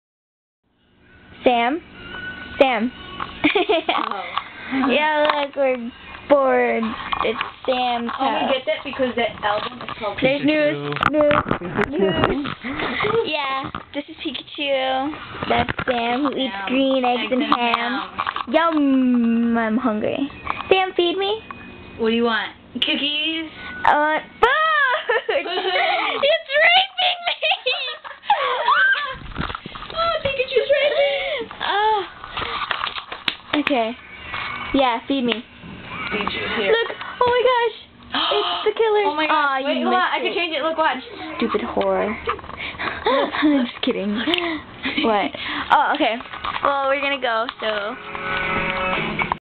pikachu randomness
pikachu humping an asian during half of the video... nuff said